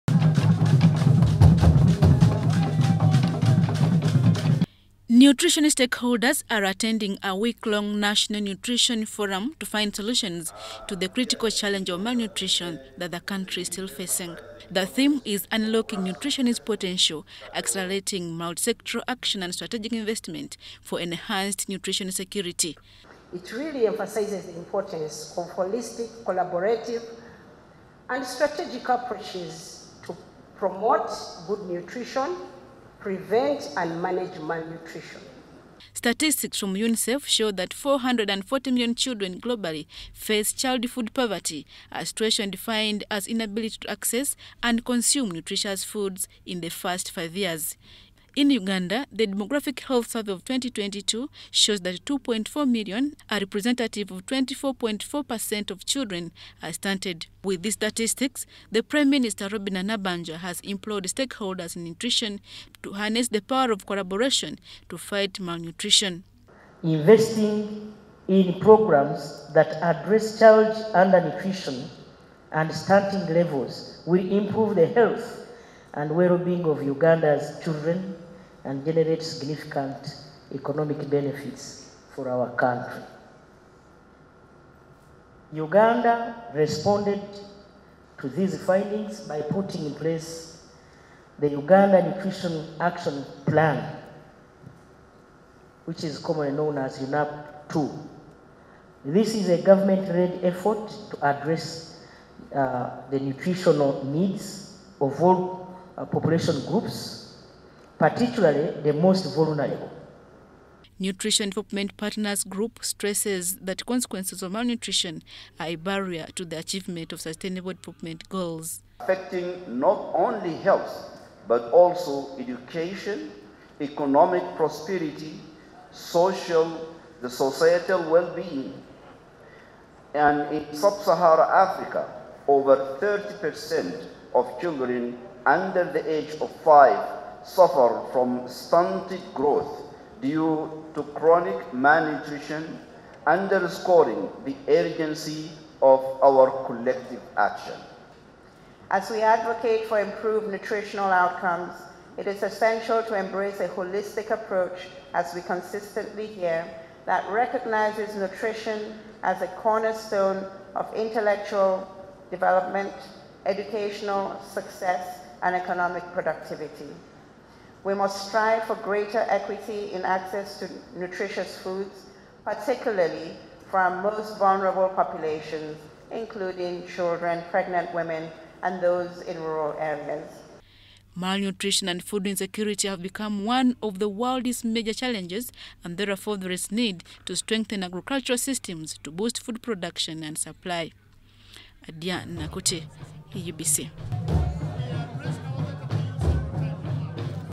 NATIONAL NUTRITION FORUM URGES INCLUSION OF NUTRITION IN DEVELOPMENT PLANS
The on-going 5-day National nutrition Forum, a platform that brings together nutrition stakeholders, has underscored the need for government to include nutrition interventions in the country’s National Development Plans.
Officiating at the forum, the Prime Minister Robinah Nabanjja conceded that malnutrition is still a challenge and implored all stakeholders to leverage on partnerships to fight to address this critical issue that affects human health and development.